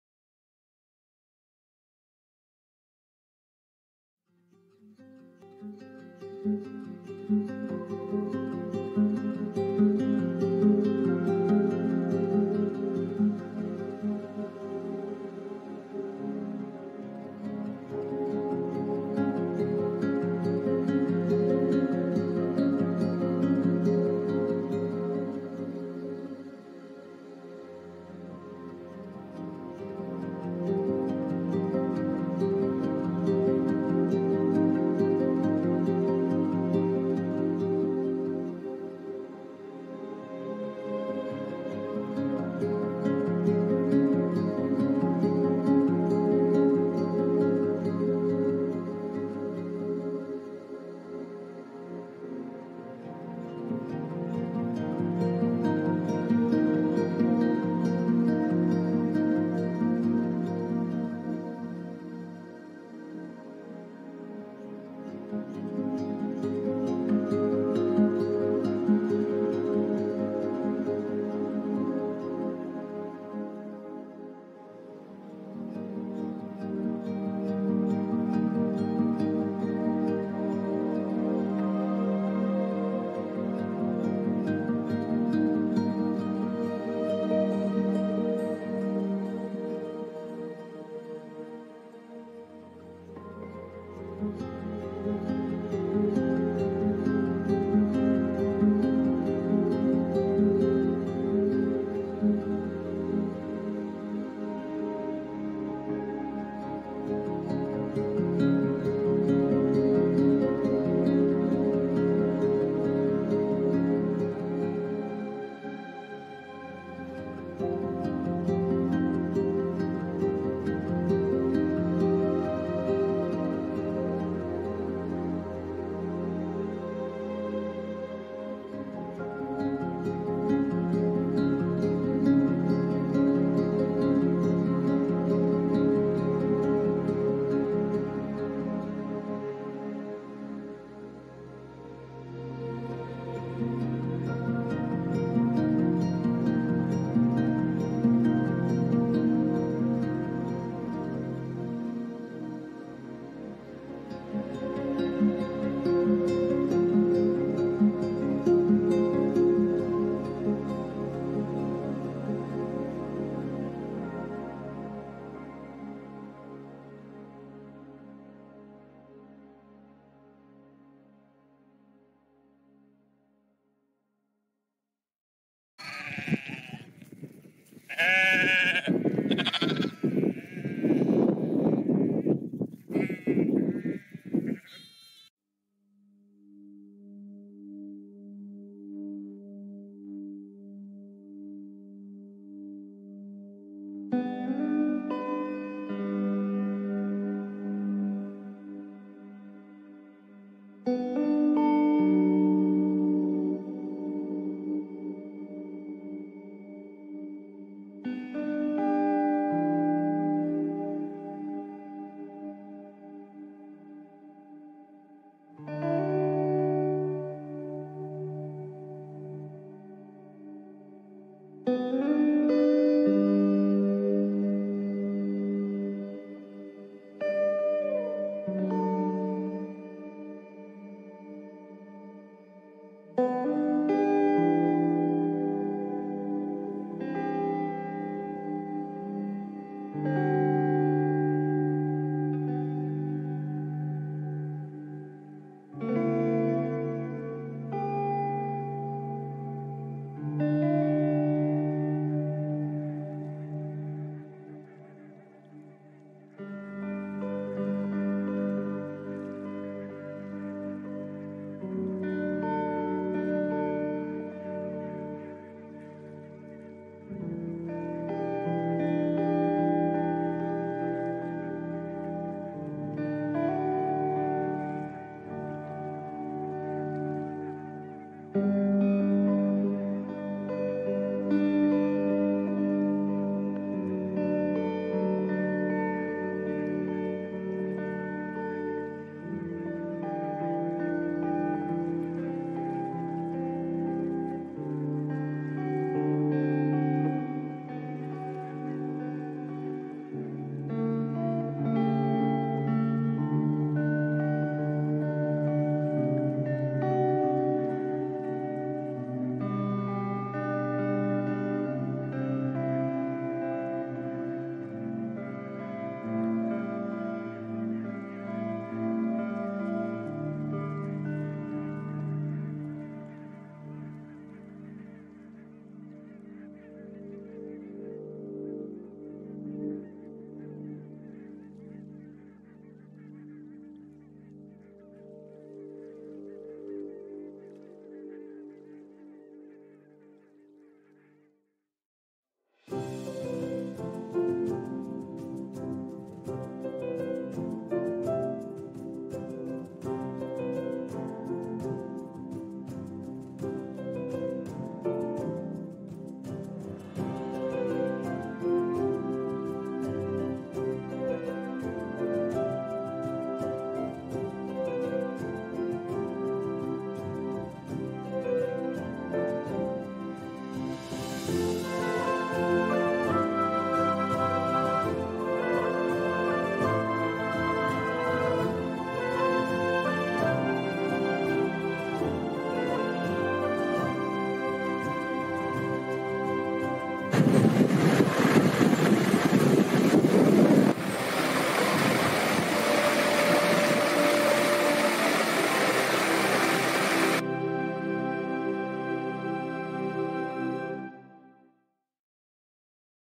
A Tour Of The Cambrian Mountains
No talking required in this video. Sit back and relax as I take you on a tour of the magnificently wild and remote Cambrian Mountains in the middle of Wales. Little known by many, there are empty moors, gorges, waterfalls and forests to be found in what is considered to be by many the last wilderness in England and Wales.
If you'd like to find out more about this very special area, then please feel free to enjoy me blog.